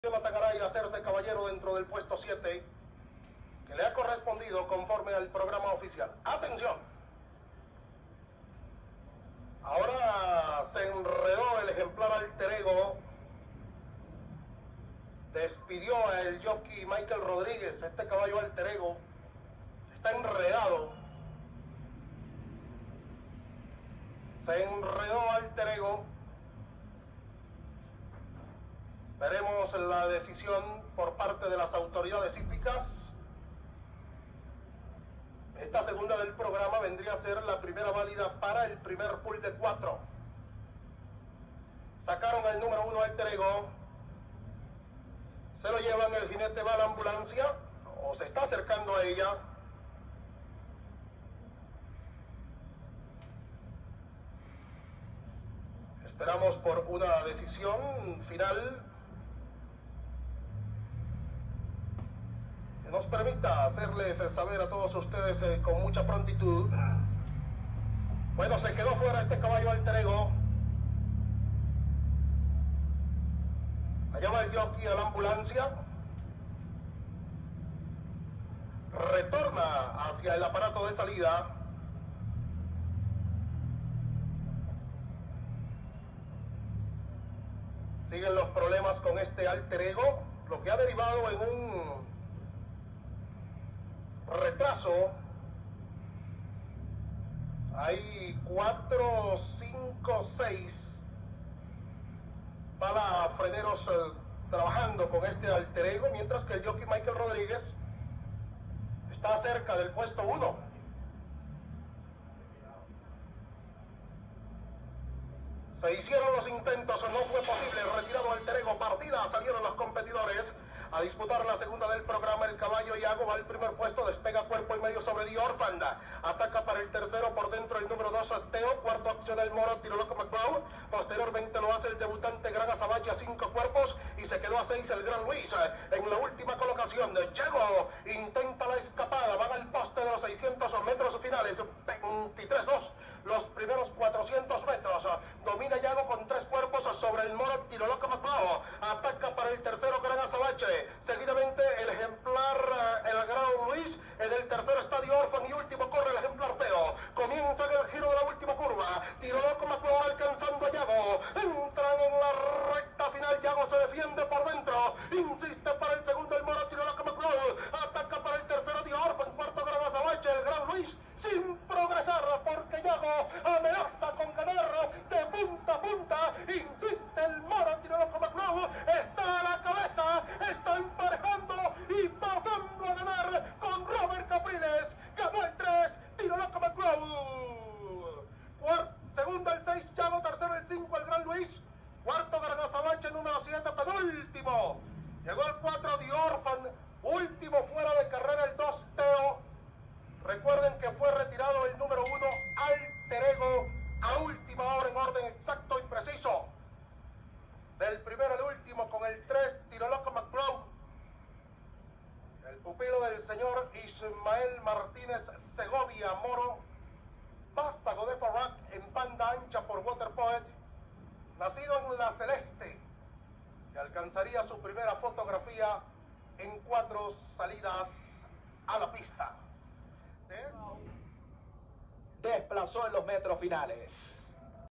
Tirolocomcgraw 🇻🇪 pasó y ganó 🏆 con Robert Capriles
Fecha: Domingo, 29 de septiembre de 2024
Tirolocomcgraw, potro hijo de, Farraj, efectivo abajo, ganó, la carrera #2 de La Rinconada 29Sep, e hizo tiempo de: 1´07”37 (1100). Montó, Robert Capriles y entrenó, Ismael Martínez. Crianza Haras La Celeste.
Video tomado de la transmisión oficial